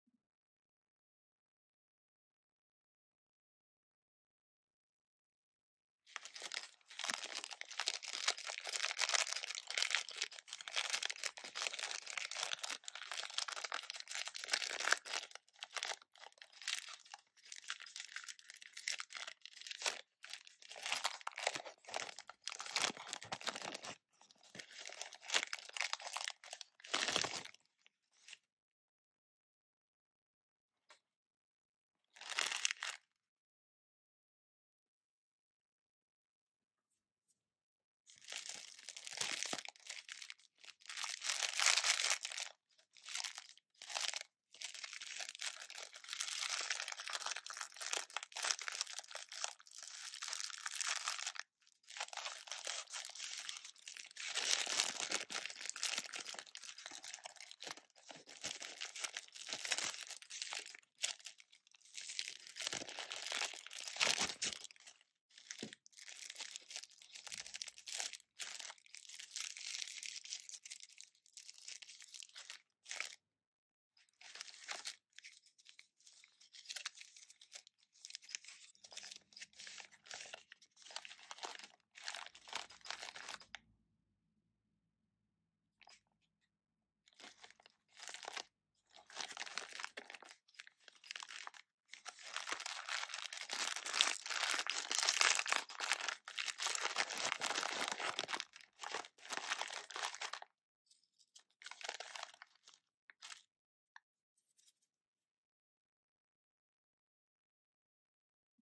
Turkish Sweets Unboxing Chocolate Cookies Candy & More!
✨ ❤️Support us for more countries, more sweets, and more chocolates, and candies!❤️ ✨

ASMR Unboxing Turkish sweets and snacks, including chocolates, cookies, candy, jelly gum, and some international brands found in Turkey!